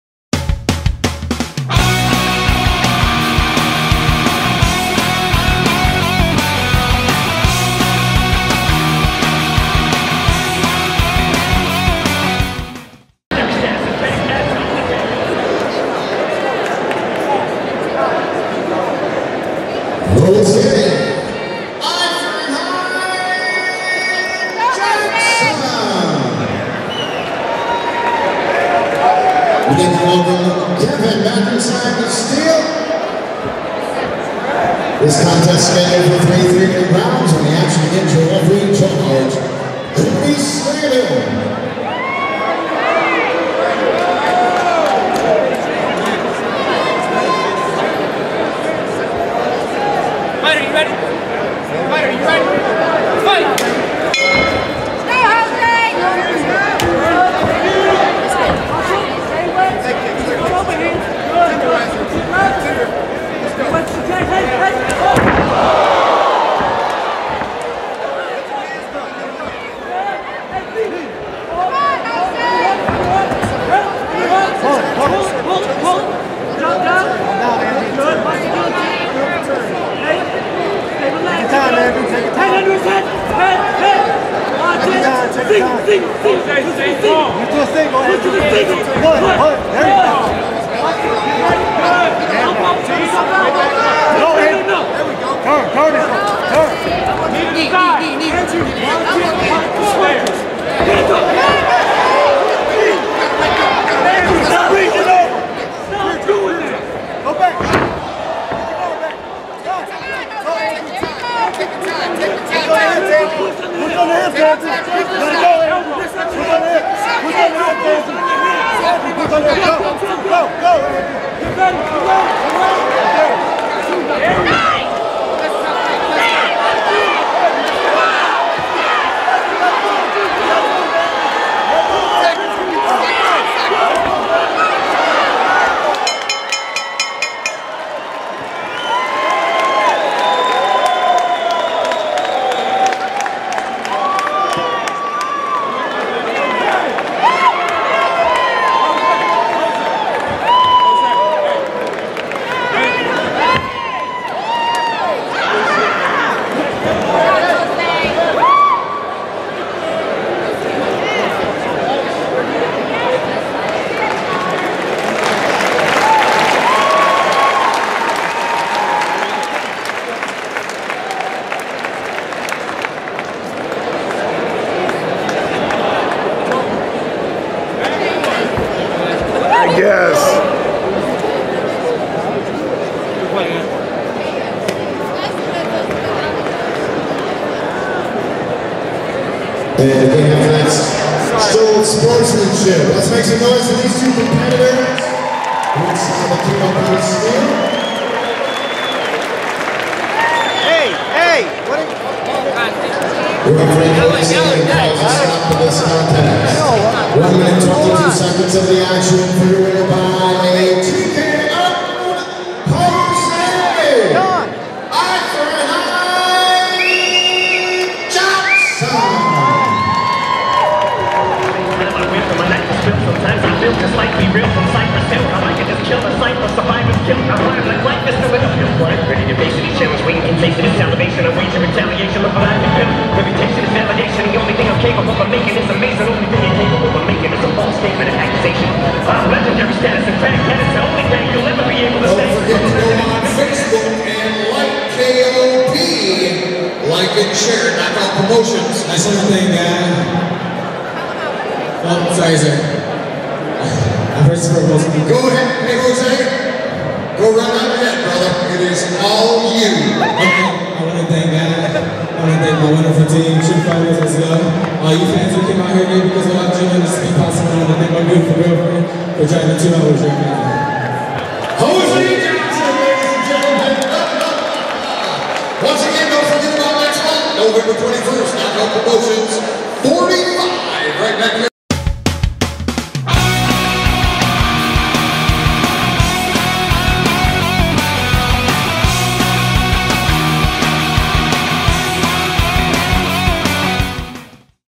"Knockout Promotions" 44 Jose Johnson vs Andrew Muyskens
ACSLIVE.TV Presents
"Knockout Promotions" 44
Jose Johnson vs Andrew Muyskens